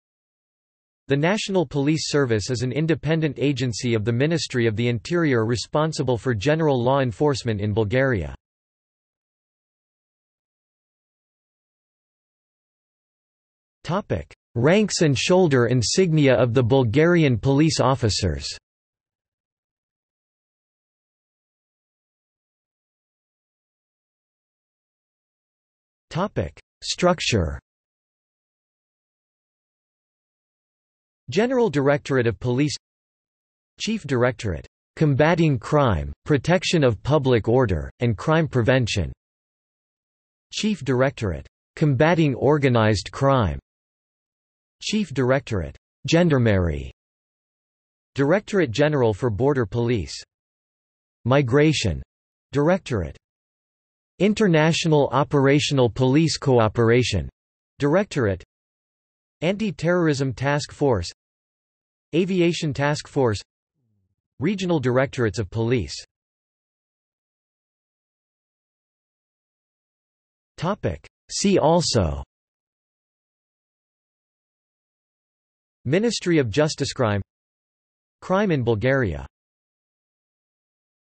National Police Service (Bulgaria) | Wikipedia audio article
This is an audio version of the Wikipedia Article:
National Police Service (Bulgaria)

00:00:10 1 Ranks and shoulder insignia of the Bulgarian police officers
00:00:22 2 Structure
00:01:07 3 See also

- Socrates

SUMMARY
The National Police Service is an independent agency of the Ministry of the Interior responsible for general law enforcement in Bulgaria.